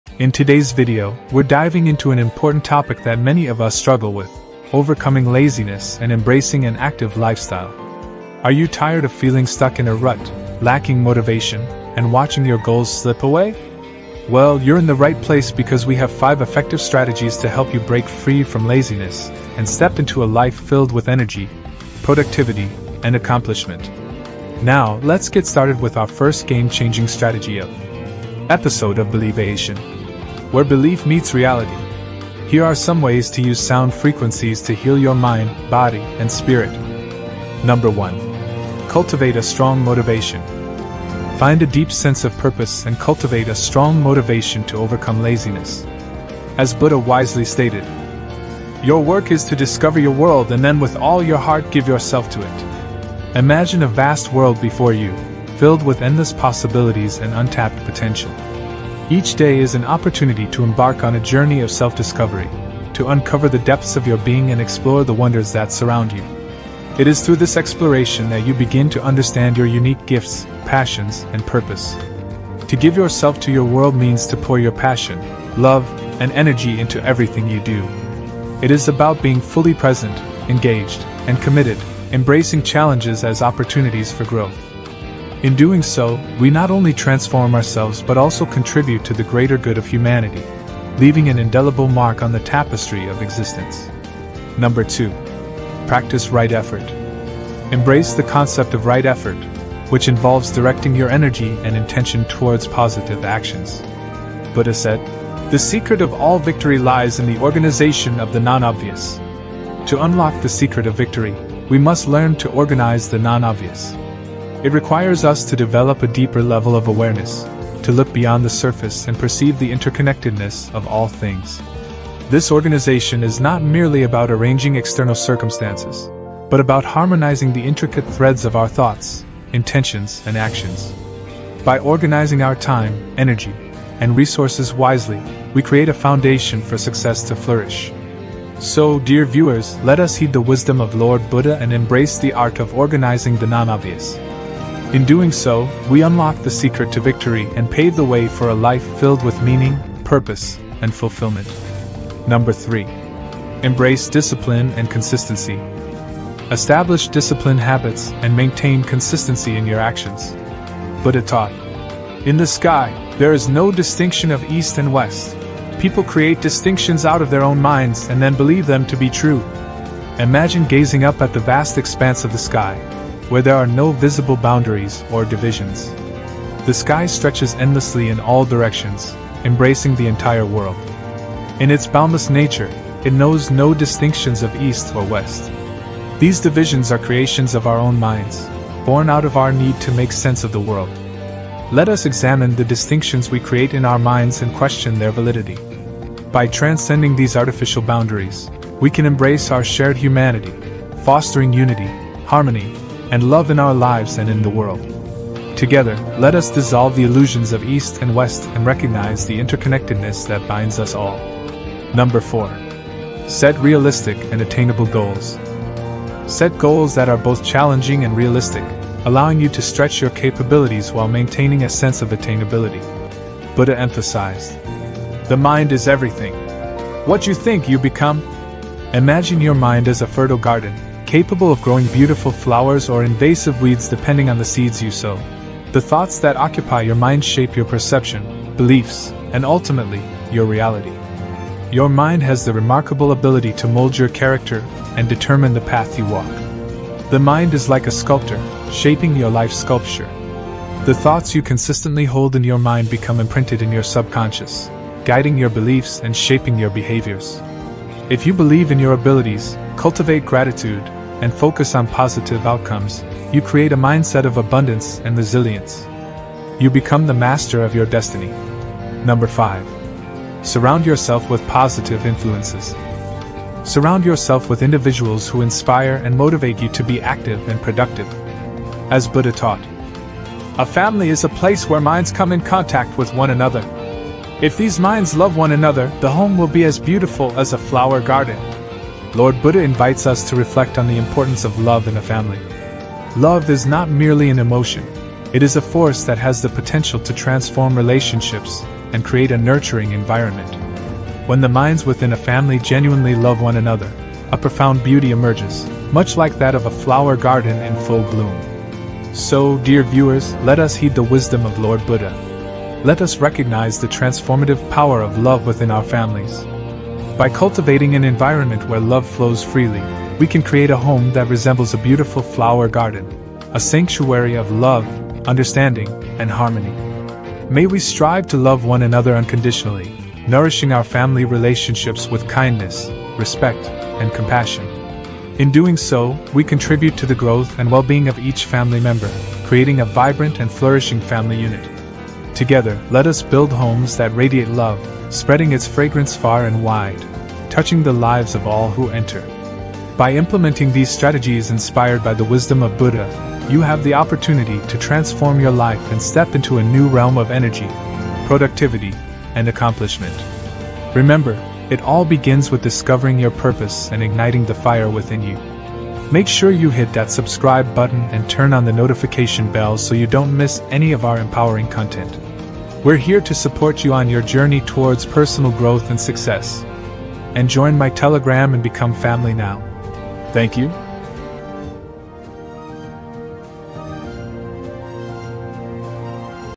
5 Japanese Technique To Overcome Laziness😎💯 Motivation
5 Japanese Technique To Overcome Laziness😎💯 Motivation

HI,
Are you ready to transform your life? In this video, we'll share 5 powerful strategies that will help you overcome laziness, ignite your motivation, and step into a life filled with energy, productivity, and accomplishment
 Do you have ambitious goals but find yourself struggling to make progress? In this exciting video, we reveal the secrets to achieving your big goals in a short period of time. Whether you're aiming for career success, personal development, or any other area of life, these proven strategies will help you fast-track your journey to success.